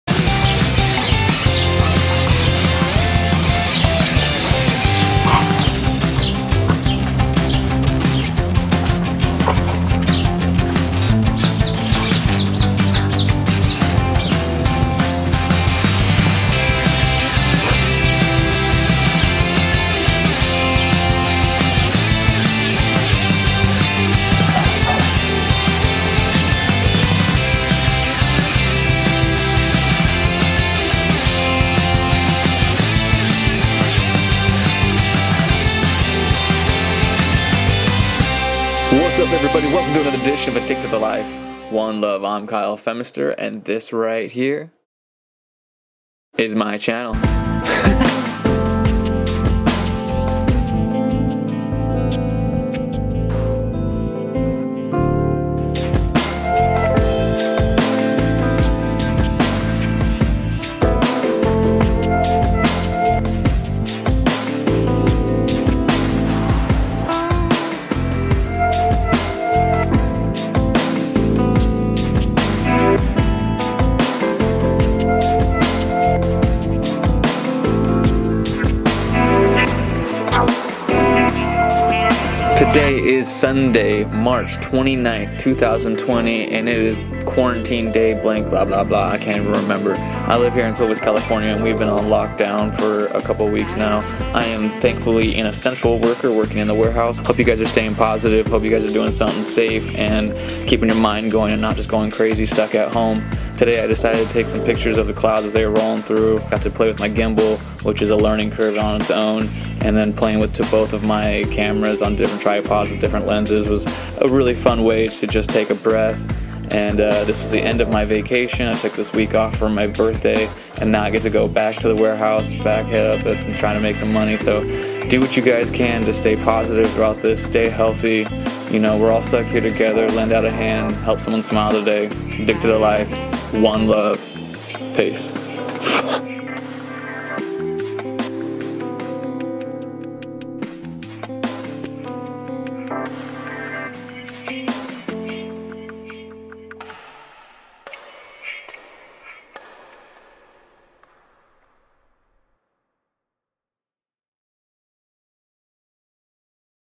Sunday Funday and this stay at home order is driving me crazy 3/29/20 Clovis California
Awesome day relaxing in my back yard watch the beautiful clouds roll by...
Stay strong and stay positive
Help Someone Smile Today
Addicted2Life 1Love .